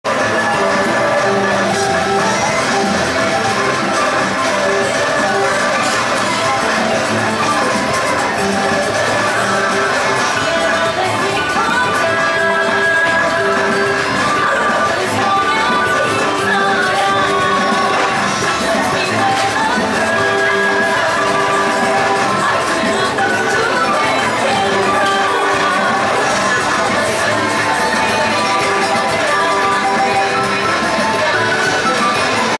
Jitotol Fair
No matter that there folkloric dancing going on, still the random fair music blasts in the background.